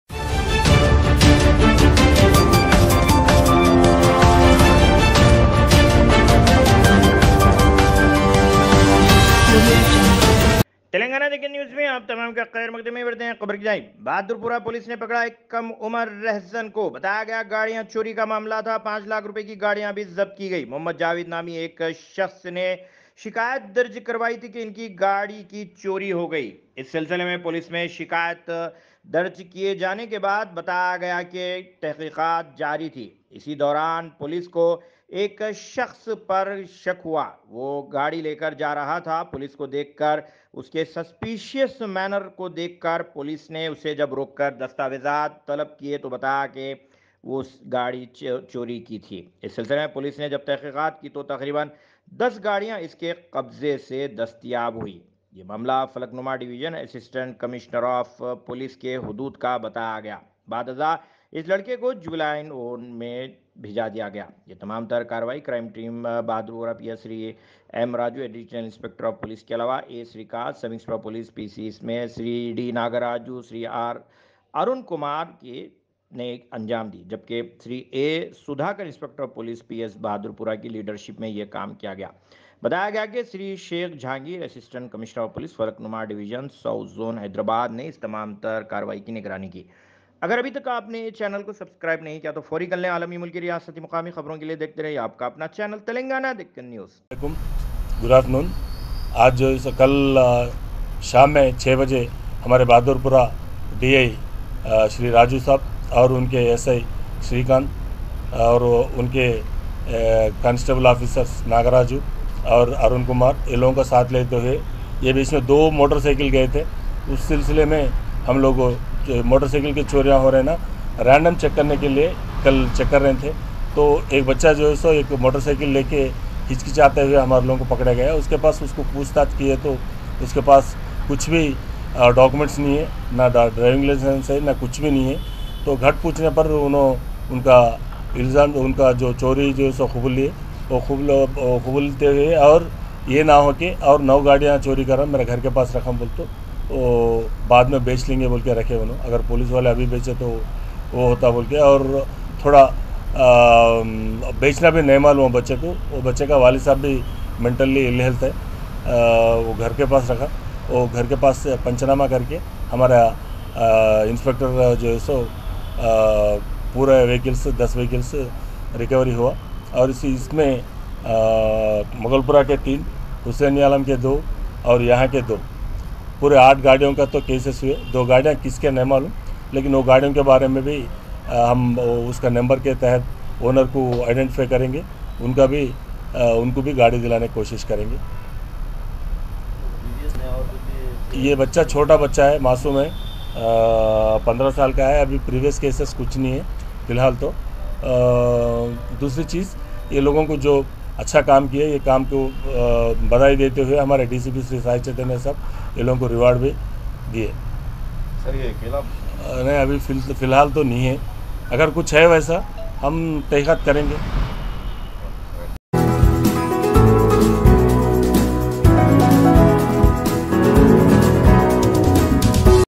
10 Bikes recovered from a Habitual Juvenile Thief by Bahadurpura Police
10 Bikes recovered from a Habitual Juvenile Thief by Bahadurpura Police